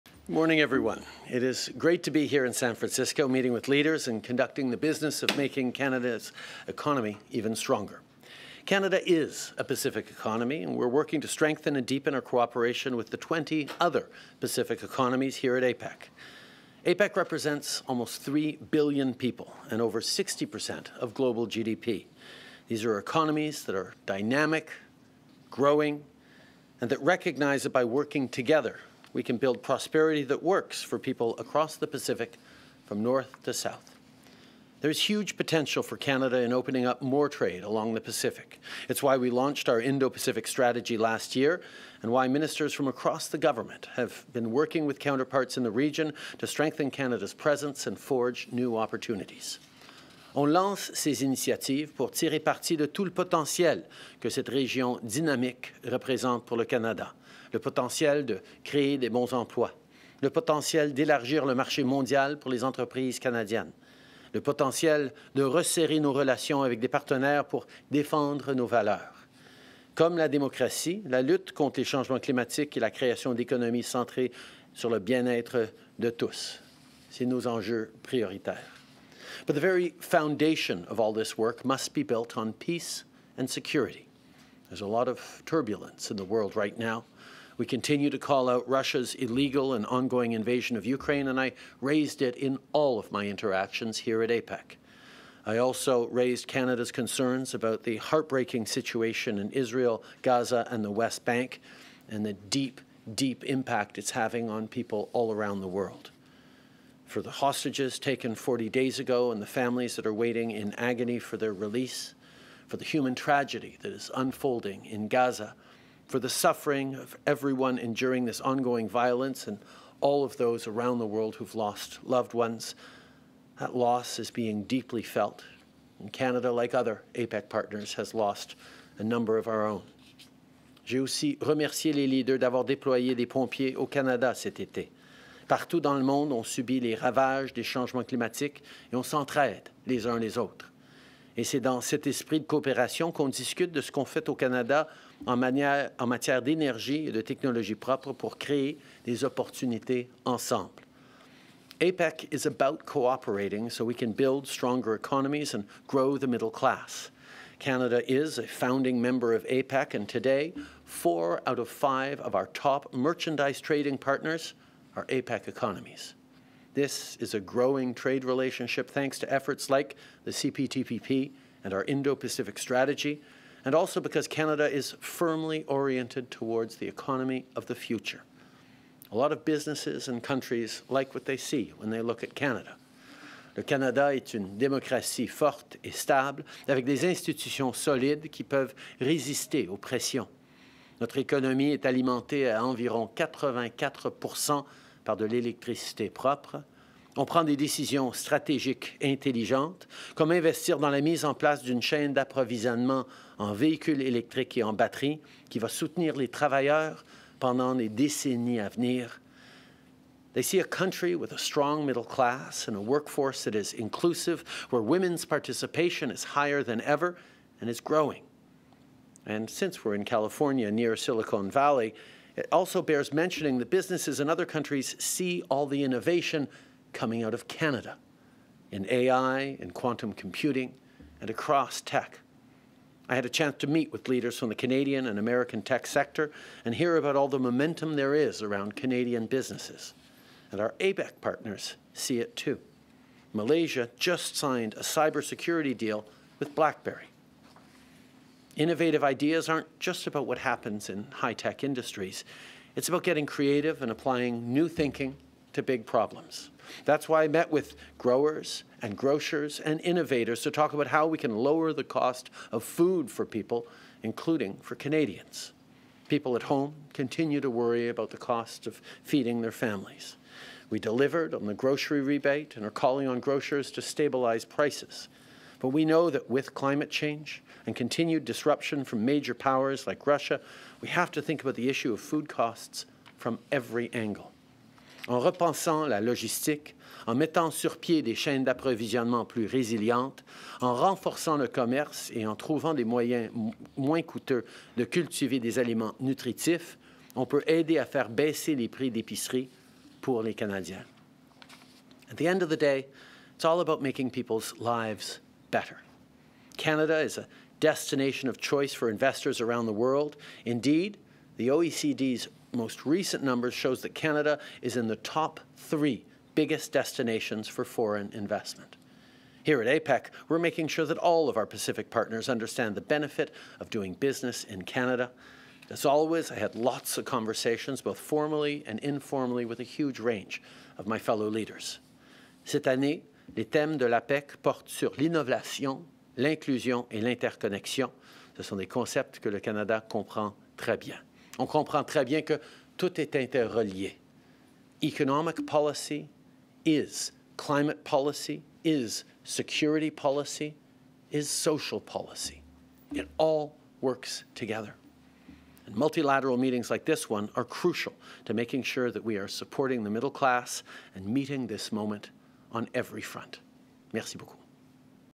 Créer des opportunités pour le Canada et renforcer la collaboration avec les partenaires de l'APEC
17 novembre 2023
Le premier ministre Justin Trudeau conclut sa participation à la Réunion des dirigeants de l'APEC à San Francisco, aux États-Unis d'Amérique, où les dirigeants de 21 économies partenaires se sont réunis pour faire progresser la coopération économique.